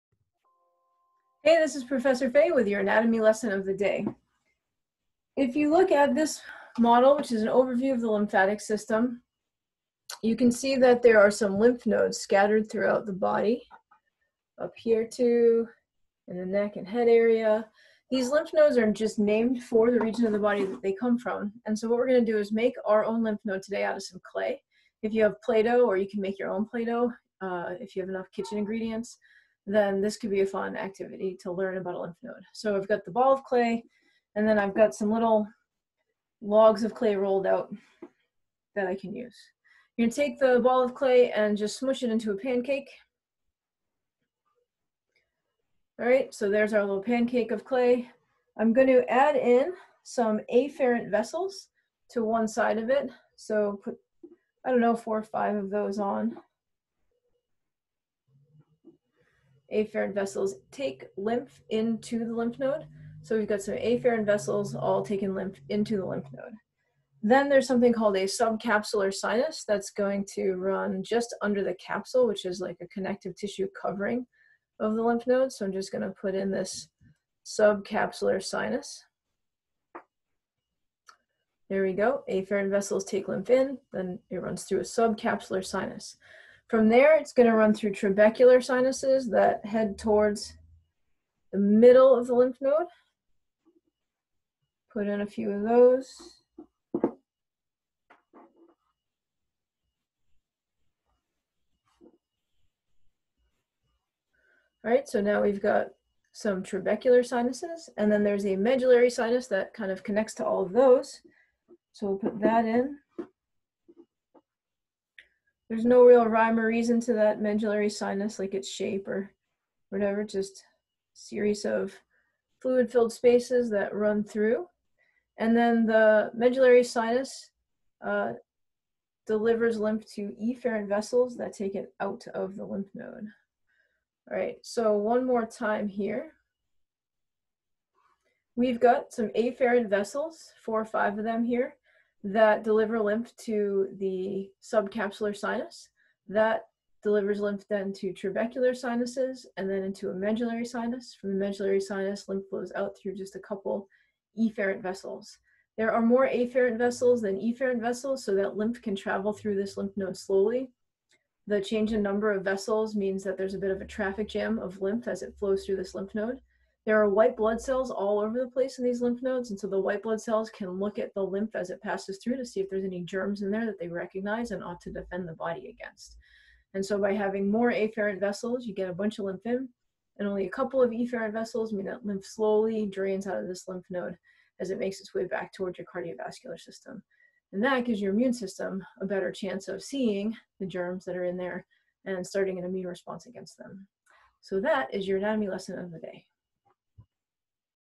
Flow of Lymph Through A Lymph Node - Anatomy In Clay
Using an Anatomy In Clay model, I point out some lymph nodes and then create a lymph node and the sinuses lymph flows though. Memory tip: "AST ME!" might help you remember that lymph flows through a lymph node in the following order: afferent vessels, subcapsular sinus, trabecular sinus, medullary sinus, and efferent vessels.  Check out the video to hear about them.

ANATOMY IN CLAY® video and building by Tara Hamilton-Fay at University of Scranton in Scranton, PA using Jon Zahourek’s MANIKEN® models.  Posted with permission and limited video use rights authorized through